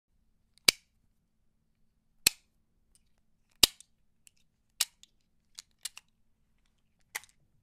Guns : Unloading Pistol Magazine - Sound Effect - FREE
Sound Effect Category:
Guns

Sound Effect Description:
Unloading each bullet from GLOCK  43 pistol.